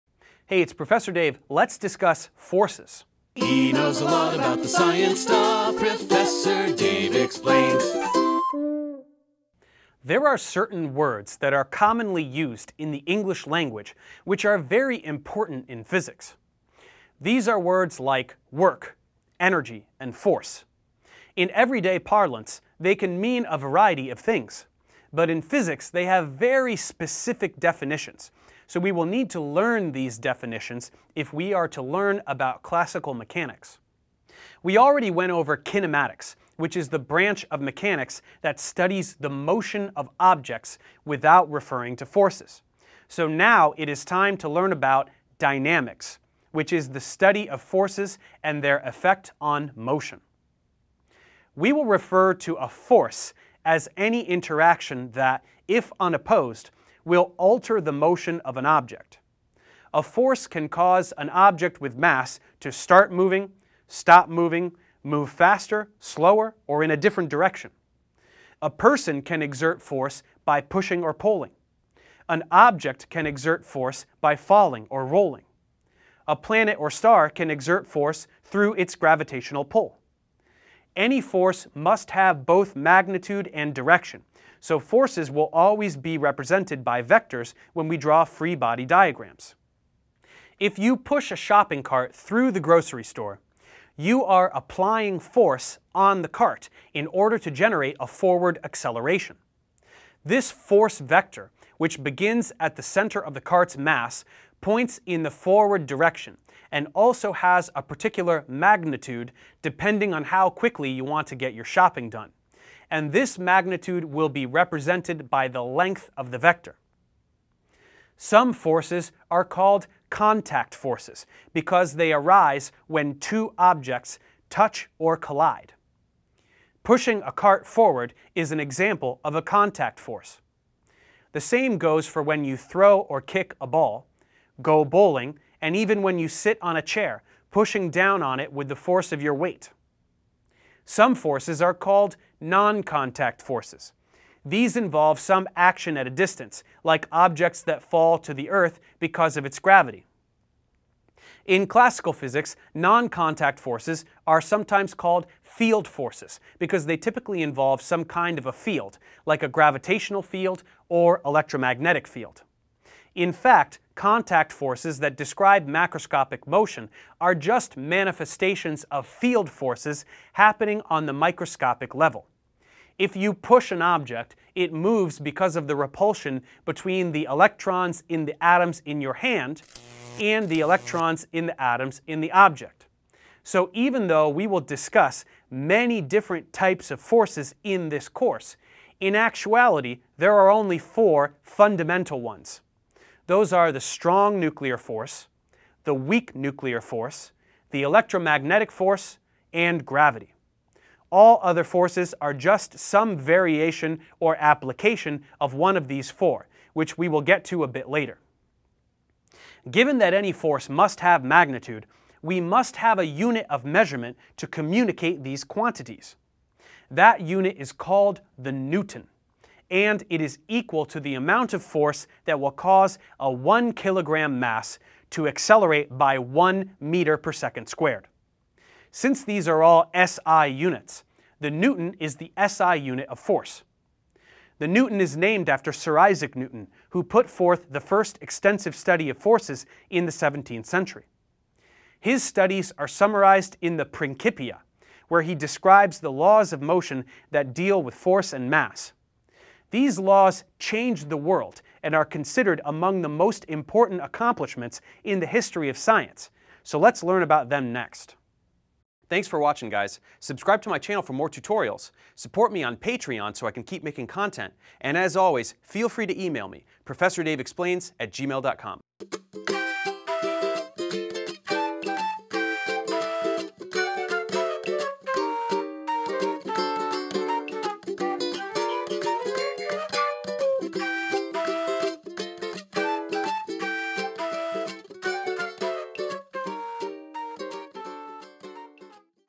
Dynamics: What are Forces?
What's a force? No, not THE force. That's from Star Wars. I'm talking about the forces in the universe that make things move. Yes, I know Yoda moved the X-Wing with The Force. Will you quit it? We are talking about the real universe here. Fine, I guess it could be the same universe as ours, but why are there humans that speak English in a galaxy far, far away? Alright fine, go watch a Star War first and then come back to me when you want to learn some real physics.